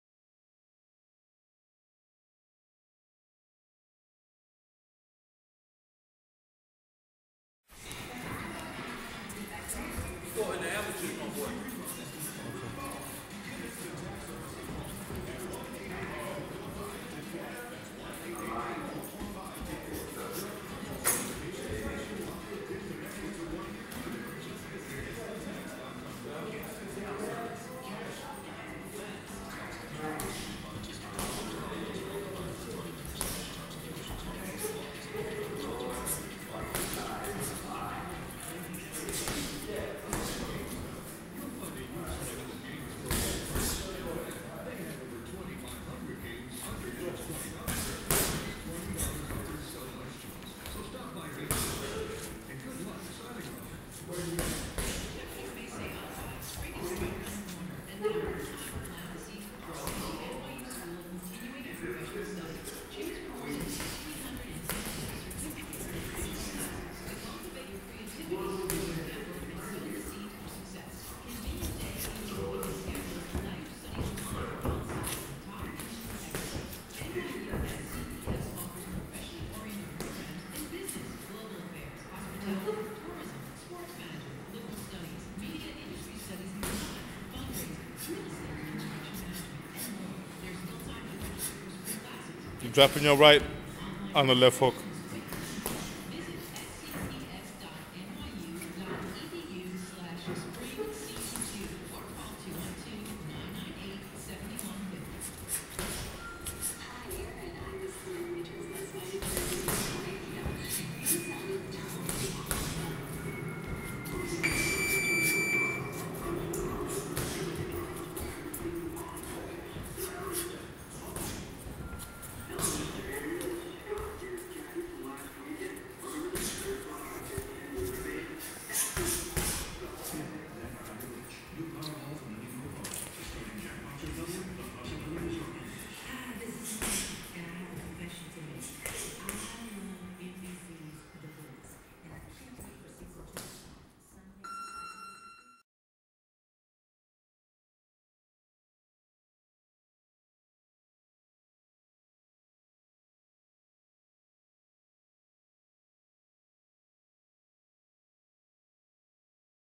the spider web
head movement and shadow boxing in the ring with ropes looking like spider web.